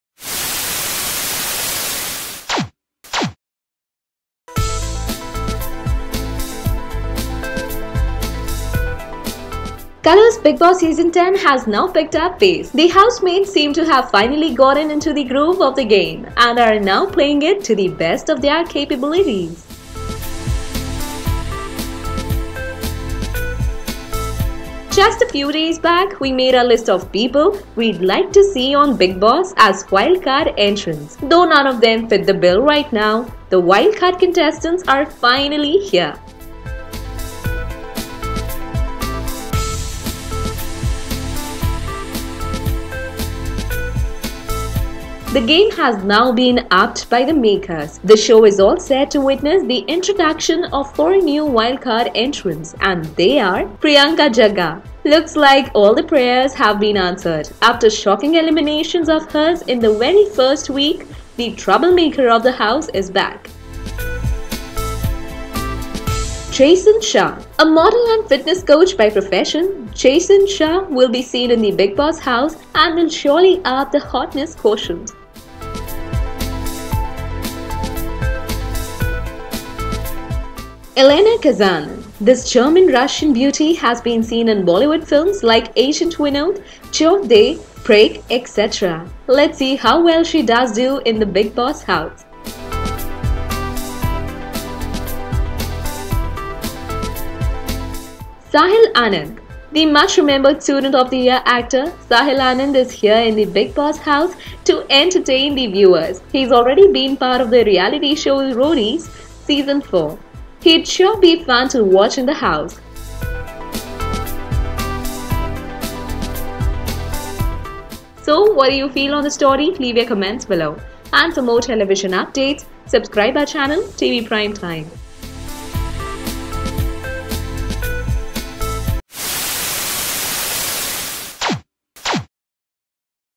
Checkout: Wild card entries | Bigg Boss 10 | TV Prime Time
It's already known that eliminated contestant Priyanka Jagga is making a comeback on Bigg Boss 10 as a wildcard entry and joining her will be three celebrities - actor Sahil Anand, German-Russian actor Elena Kazan and international model Jason Shah. The three names might not be popular but going by their profiles, it is certain that they will bring a lot of drama to the controversial reality show.